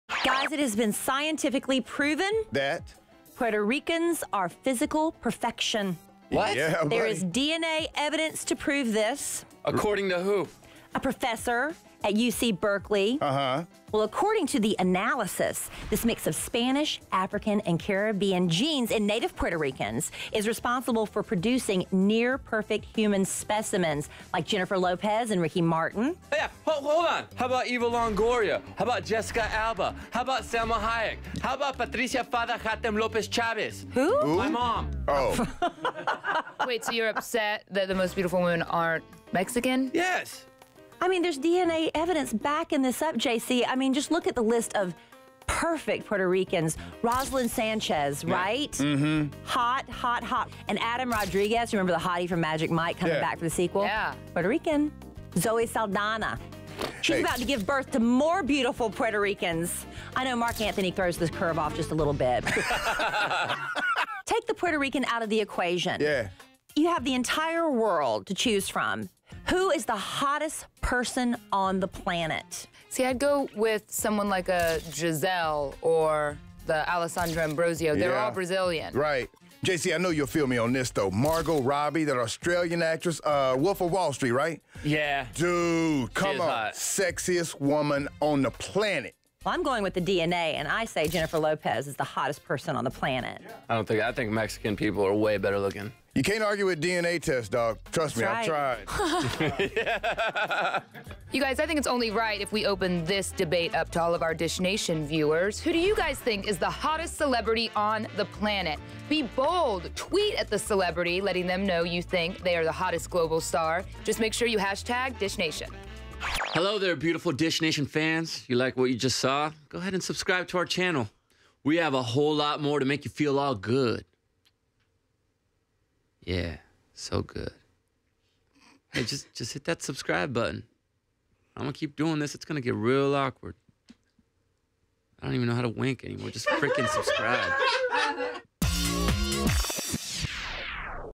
Are Puerto Ricans the Perfect Humans?
Do Puerto Ricans have the best genes? Well, we can't really argue with Jennifer Lopex, Ricky Martin and Zoe Saldana! Who do you think the hottest celeb is?